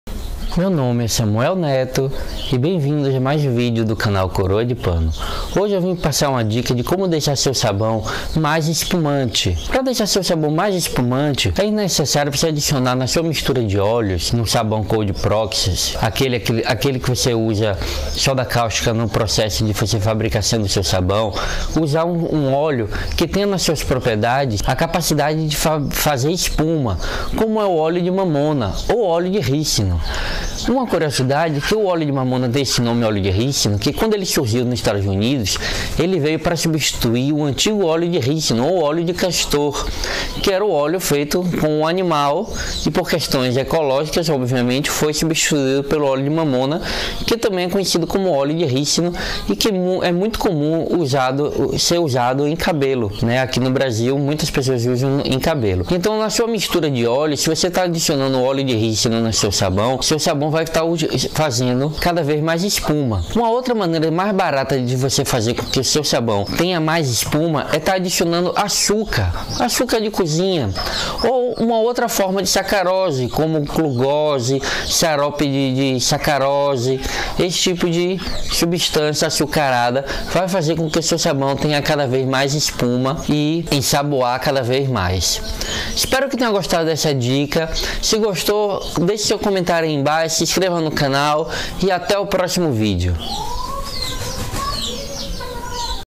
Como fazer o sabão fazer mais espuma pelo método do Cold Process: dicas e truques
Se você é um amante do sabão artesanal e deseja obter uma espuma mais abundante e duradoura, este vídeo é para você! Aqui, você aprenderá as melhores dicas e truques para aumentar a espuma do seu sabão fabricado pelo método do Cold Process.

Ao longo do vídeo, você descobrirá como escolher os ingredientes corretos para obter uma espuma mais densa e cremosa, além de aprender a adicionar ingredientes especiais que ajudam a aumentar a produção de espuma. Também irá aprender a controlar a temperatura e o tempo de cura para maximizar o potencial de espuma do seu sabão.

Além disso, você receberá dicas sobre a utilização de corantes e óleos essenciais que podem contribuir para a aparência e aroma do sabão, bem como para a produção de espuma. Também irá descobrir como evitar erros comuns que podem impedir a produção de uma espuma satisfatória.

Assista agora a este vídeo para aprender todas as dicas e truques para fazer o sabão fazer mais espuma pelo método do Cold Process e elevar a qualidade de suas produções de sabão artesanal!